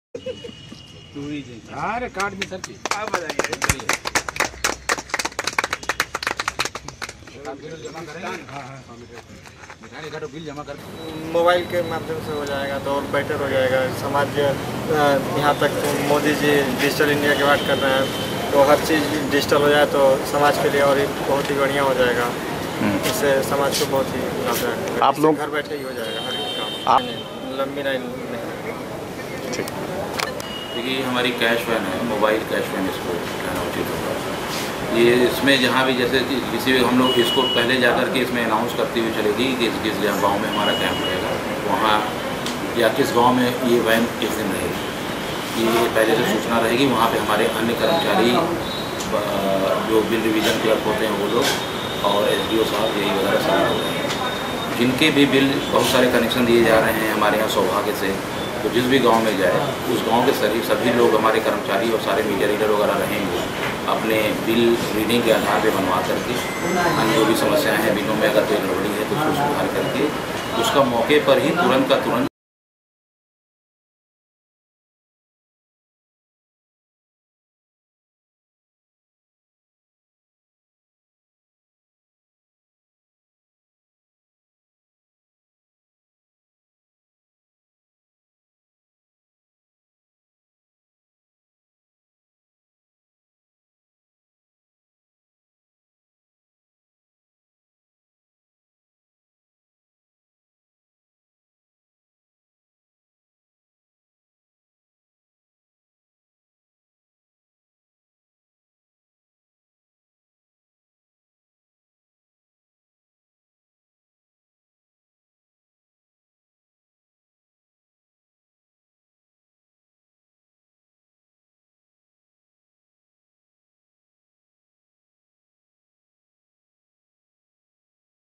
बिजली बिल जमा करने को नहीं लगानी होगी लाइन, घर-घर जाएगी मोबाइल वैन
गाजीपुर : बिजली उपभोक्ताओं को अब लम्बी लाइनों में खड़े होकर बिजली बिल जमा नहीं करना पड़ेगा. पूर्वांचल विद्युत वितरण निगम के उपकेंद्र के साथ-साथ अब मोबाइल वैन में भी उपभोक्ताओं को विद्युत बिल जमा करने की सुविधा मिलेगी. जिले में मोबाइल वैन को हरी झंडी दिखाकर रवाना किया गया है. यह वैन पूरे जिले में दौड़ेगी. इससे राजस्व में भी बढ़ोतरी होगी.